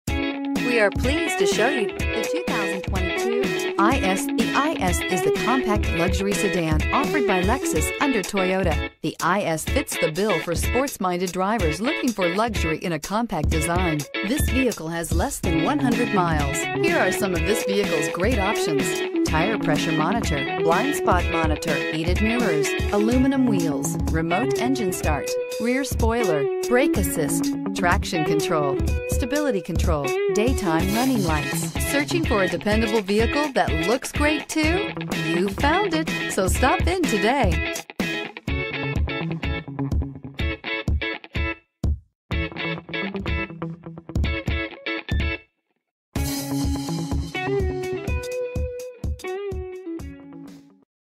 2022 Lexus IS N31904
2022 Lexus IS IS 350 F SPORT

Pick up this Atomic Silver 2022 Lexus IS, available today at Lexus of West Kendall. This could be the one you've been searching for.